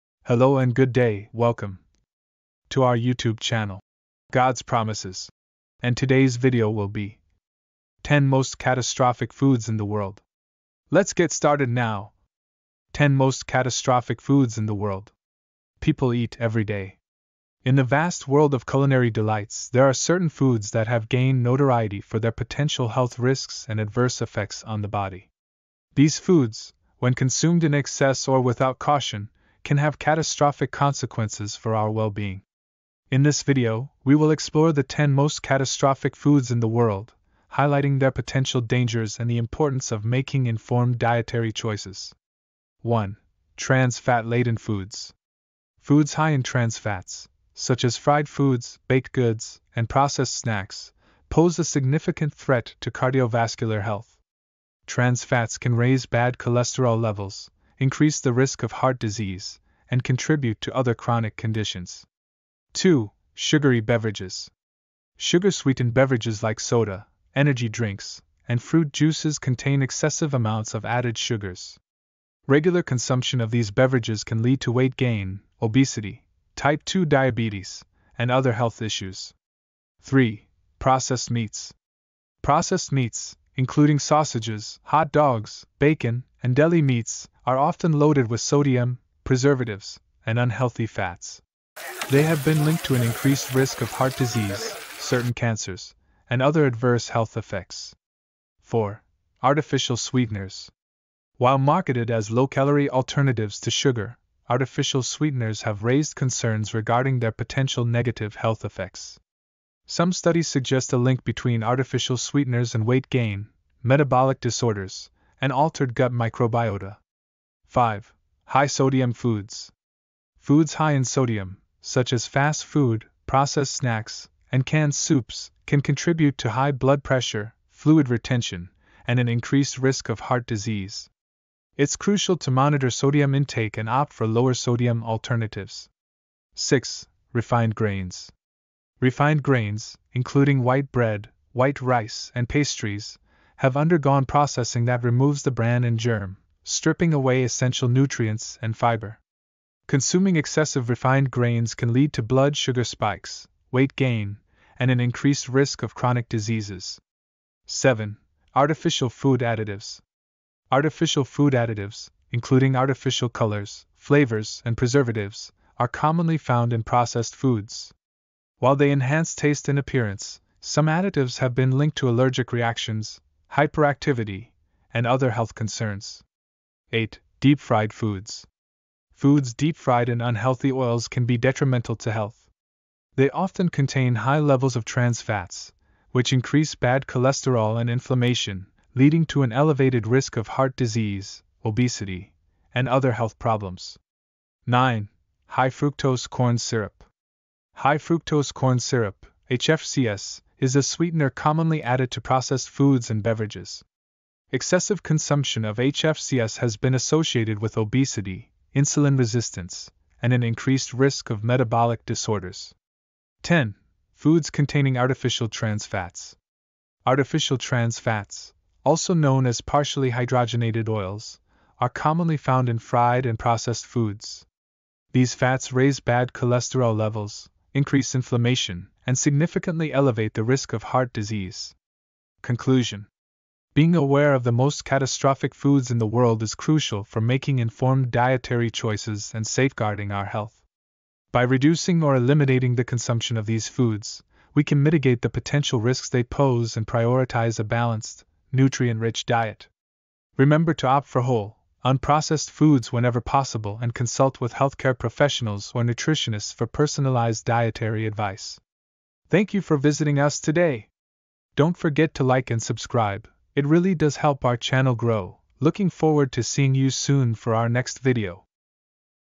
10 Most Catastrophic Foods In The World People Eat Everyday
10 Most Catastrophic Foods In The World People Eat Everyday

Hello and Good Day, Welcome, To Our YouTube Channel "God's Promises", and Today's Video Will Be: 10 Most Catastrophic Foods In The World "Lets Get Started Now" 10 Most Catastrophic Foods in the World People Eat Everyday.  In the vast world of culinary delights, there are certain foods that have gained notoriety for their potential health risks and adverse effects on the body. These foods, when consumed in excess or without caution, can have catastrophic consequences for our well-being. In this video, we will explore the ten most catastrophic foods in the world, highlighting their potential dangers and the importance of making informed dietary choices. 1. Trans Fat Laden Foods Foods high in trans fats, such as fried foods, baked goods, and processed snacks, pose a significant threat to cardiovascular health. Trans fats can raise bad cholesterol levels, increase the risk of heart disease, and contribute to other chronic conditions. 2. Sugary Beverages Sugar-sweetened beverages like soda, energy drinks, and fruit juices contain excessive amounts of added sugars. Regular consumption of these beverages can lead to weight gain, obesity, type 2 diabetes, and other health issues. 3. Processed Meats Processed meats, including sausages, hot dogs, bacon, and deli meats, are often loaded with sodium, preservatives, and unhealthy fats. They have been linked to an increased risk of heart disease, certain cancers, and other adverse health effects. 4. Artificial Sweeteners While marketed as low-calorie alternatives to sugar, artificial sweeteners have raised concerns regarding their potential negative health effects. Some studies suggest a link between artificial sweeteners and weight gain, metabolic disorders, and altered gut microbiota. 5. High-Sodium Foods Foods high in sodium, such as fast food, processed snacks, and canned soups, can contribute to high blood pressure, fluid retention, and an increased risk of heart disease. It's crucial to monitor sodium intake and opt for lower-sodium alternatives. 6. Refined Grains Refined grains, including white bread, white rice, and pastries, have undergone processing that removes the bran and germ, stripping away essential nutrients and fiber. Consuming excessive refined grains can lead to blood sugar spikes, weight gain, and an increased risk of chronic diseases. 7. Artificial Food Additives Artificial food additives, including artificial colors, flavors, and preservatives, are commonly found in processed foods. While they enhance taste and appearance, some additives have been linked to allergic reactions, hyperactivity, and other health concerns. 8. Deep-Fried Foods Foods deep-fried in unhealthy oils can be detrimental to health. They often contain high levels of trans fats, which increase bad cholesterol and inflammation, leading to an elevated risk of heart disease, obesity, and other health problems. 9. High-Fructose Corn Syrup High-fructose corn syrup (HFCS) is a sweetener commonly added to processed foods and beverages. Excessive consumption of HFCS has been associated with obesity, insulin resistance, and an increased risk of metabolic disorders. 10. Foods Containing Artificial Trans Fats Artificial trans fats, also known as partially hydrogenated oils, are commonly found in fried and processed foods. These fats raise bad cholesterol levels, increase inflammation, and significantly elevate the risk of heart disease. Conclusion Being aware of the most catastrophic foods in the world is crucial for making informed dietary choices and safeguarding our health. By reducing or eliminating the consumption of these foods, we can mitigate the potential risks they pose and prioritize a balanced, nutrient-rich diet. Remember to opt for whole, unprocessed foods whenever possible and consult with healthcare professionals or nutritionists for personalized dietary advice.